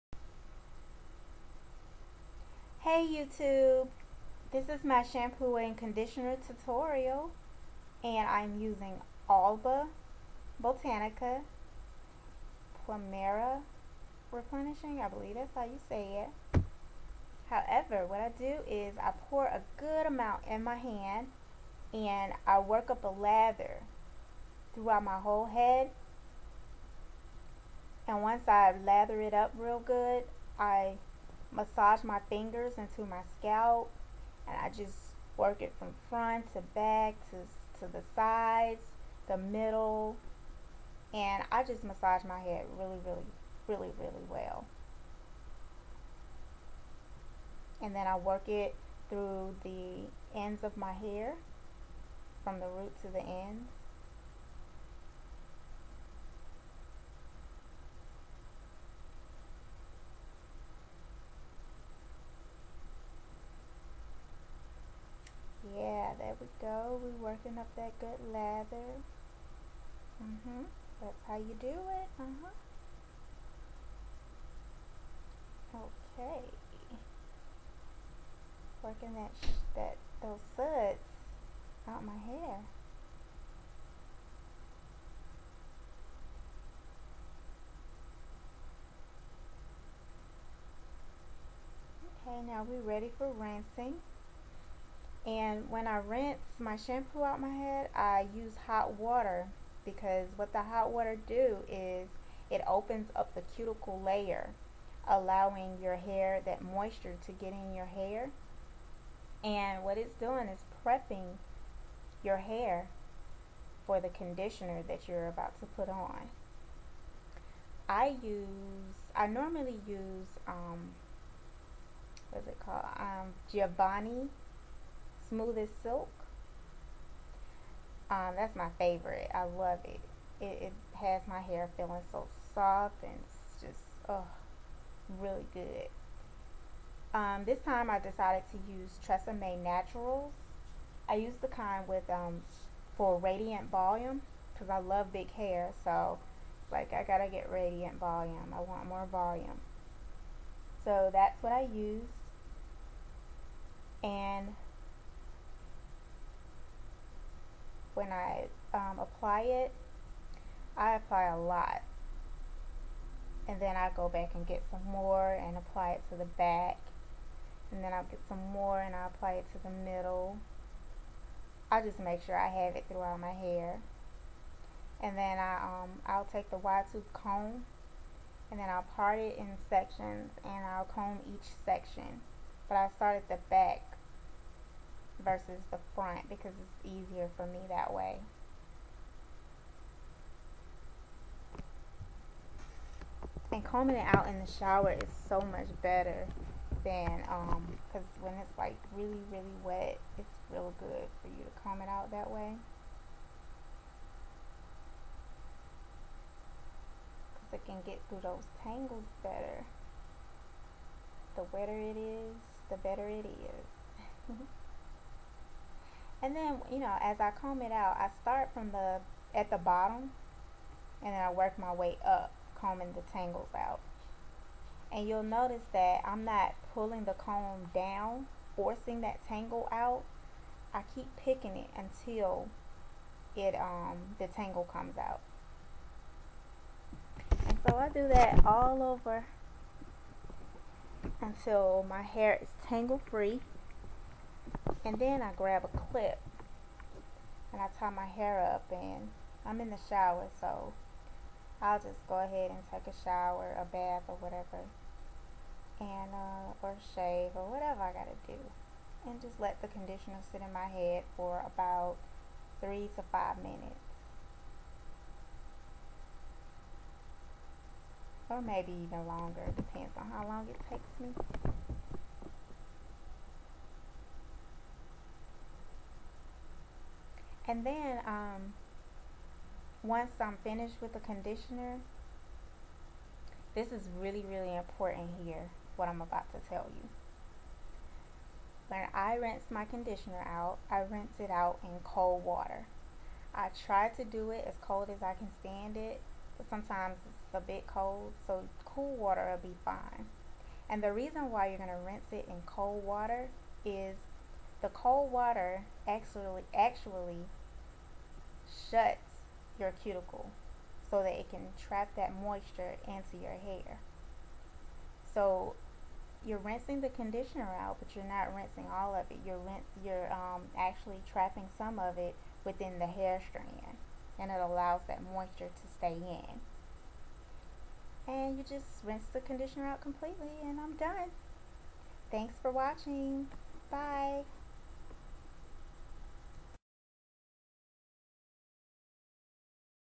My Shampoo & Conditioner Routine
Here I demonstrate how I normally wash and condition my hair in the shower. These are just techniques I use and what works for me. You can choose to try it if you like it and if you're already doing it, then great! Keep doing it!

Products used
Shampoo: Alba replenishing plumeria
Conditioner: Tressemme Naturals radiant volume

I love using Giovanni's smooth as silk deep conditioner, however, I use a lot of conditioner and it can be expensive. So I will give cheap conditioners a try and as soon as I find one that gives me alot of slipp, I'm sticking to it.